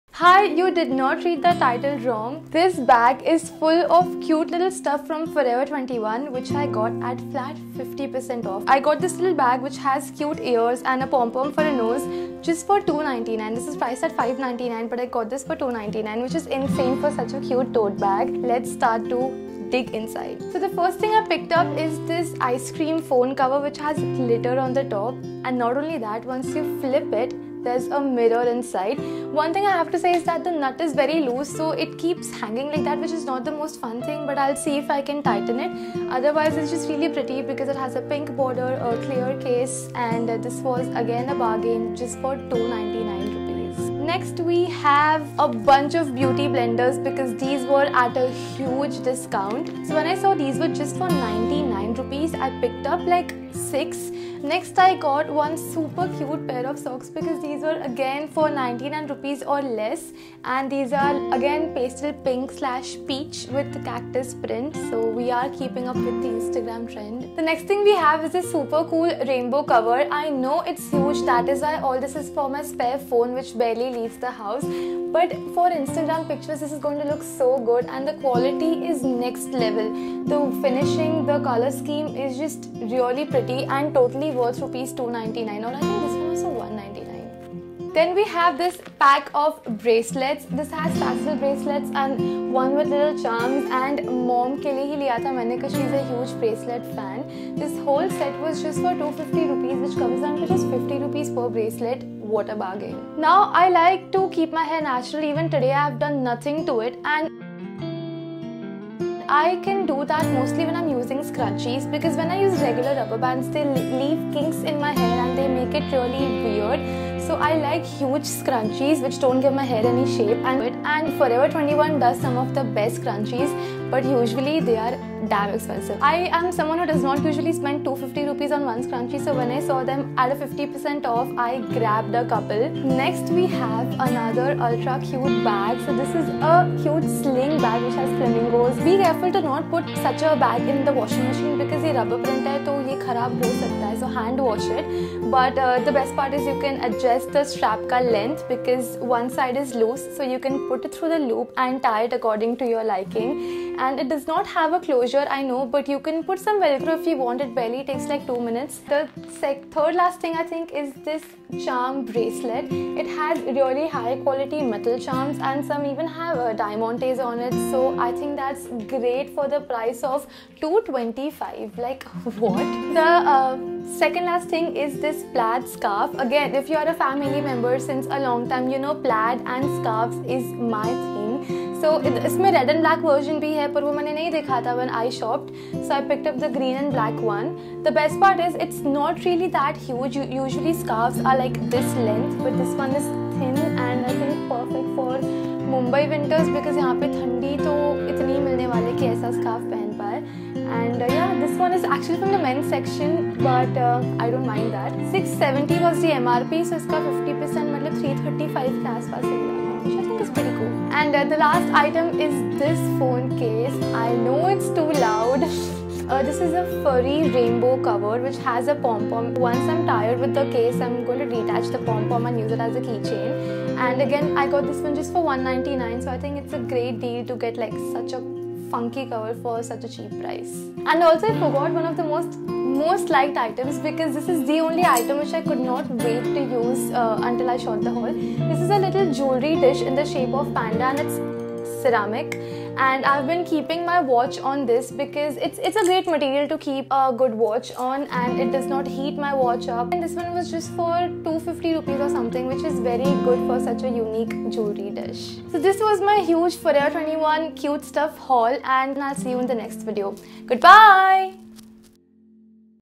*HUGE* UNUSUAL Forever21 Haul! Everything Under Rs 500 - FLAT 50% OFF
Guys, the sale is over now but I will leave the links to the products here incase they have a sale again! :))

Teardrop Makeup Sponge

Flamingo Graphic Tote

Pom Pom Case for iPhone 6/6S

Ice Cream Case for iPhone 6/6s

Love,
Heli ;)